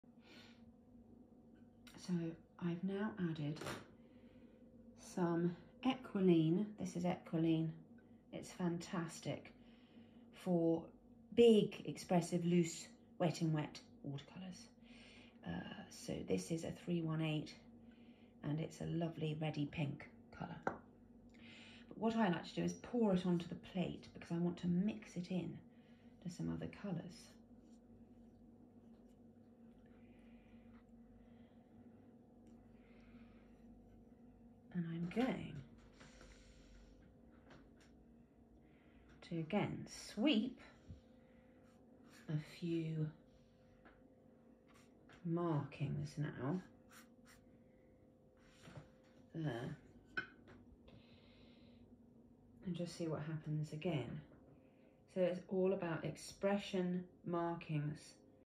Control of watercolour ink with Samantha
I am slightly dyslexic and meant to call the watercolour ink ECOLINE they have a great range of colours and you can be very expressive. I like to pour some ink onto my china plate and then I mix paint from a tube into it. I am using a red ink which I mix into the blues that I have used, Making sure the paper is moist I sweep in areas over my sky using my Chinese hake flat keep your hand taught your wrist relaxed be expressive and do shorter sweeps for the clouds.